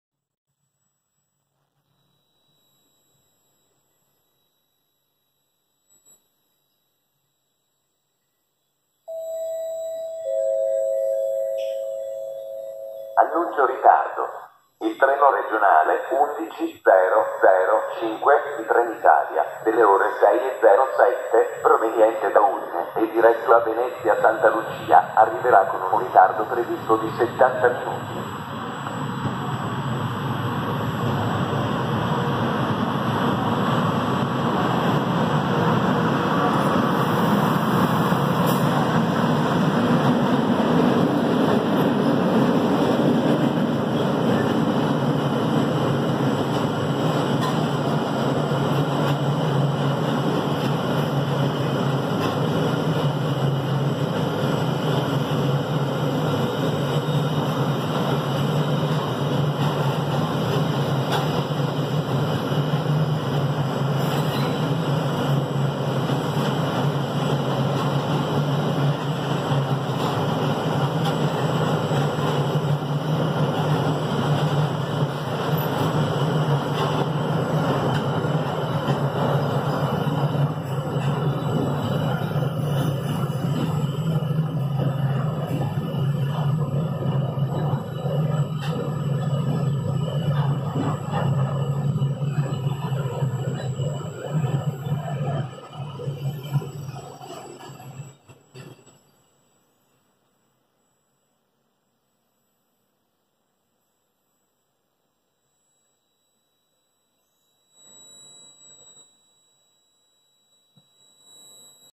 Tigrotto con carri porta furgoni Treviso 18/04/2019
Tigrotto con carri pianali porta furgoni,in transito a Treviso
Le E.632/E.633 sono una serie di locomotori elettrici multiruolo costruiti dalle Ferrovie dello Stato italiane all'inizio degli anni ottanta e tuttora in servizio e sono soprannominate "Tigri". Da esse è derivato il gruppo E.620, una versione a 4 assi usata dalle Ferrovie Nord Milano, soprannominata "Tigrotto". Wikipedia
Diametro ruote motrici: 1 040 mm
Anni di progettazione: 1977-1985
Rapporto di trasmissione: 36/64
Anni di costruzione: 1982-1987
Anni di esercizio: Dal 1983
Massa in servizio: 103 t
Velocità massima omologata: 160 km/h